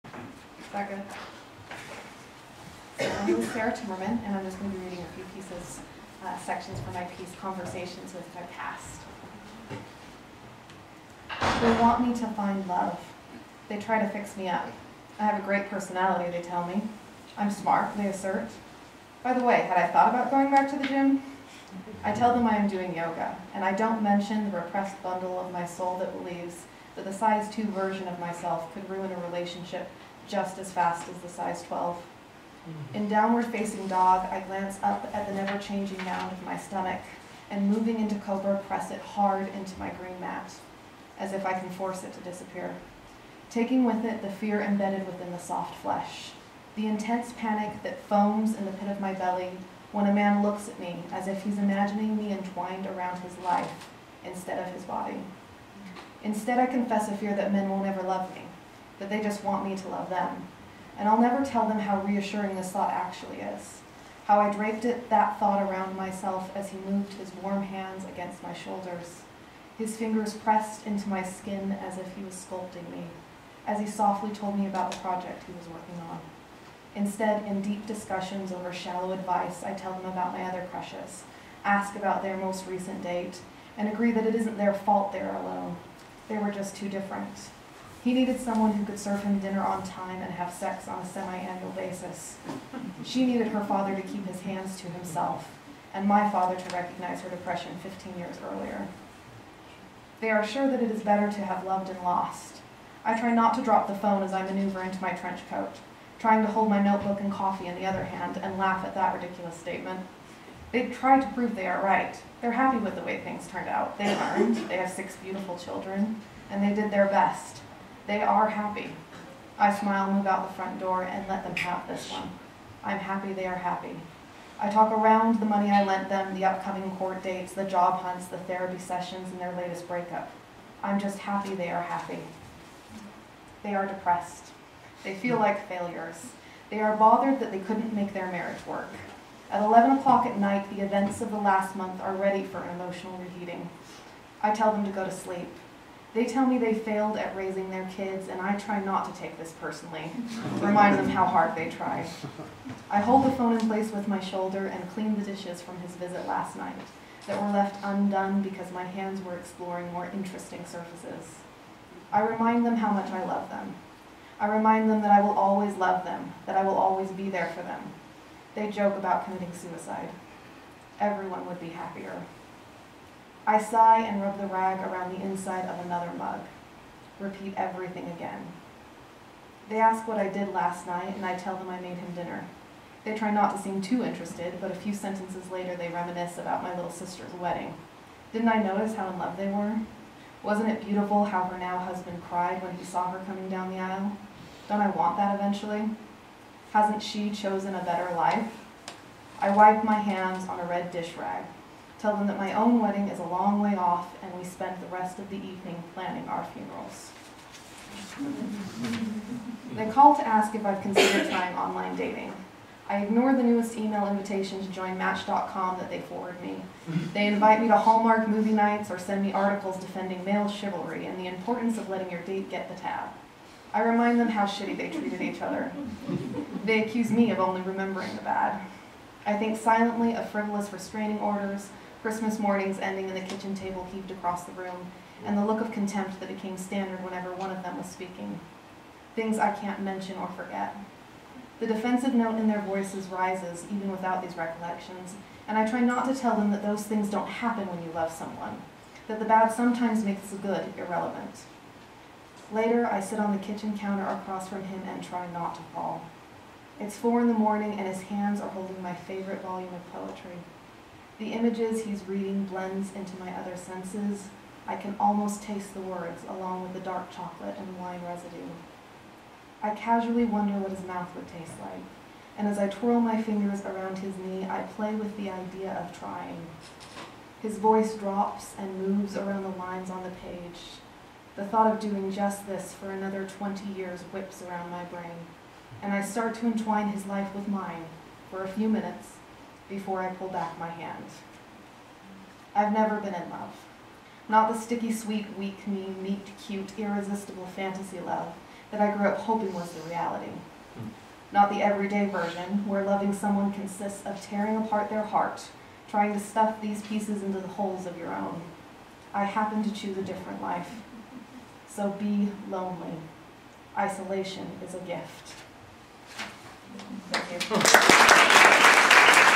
Helicon West 4/23/15: Sarah Timmerman
Sarah Timmerman performs during Helicon West at the Logan Library April 23, 2015.
Featured readers: Scribendi Winners
All events are free, uncensored, and open to the public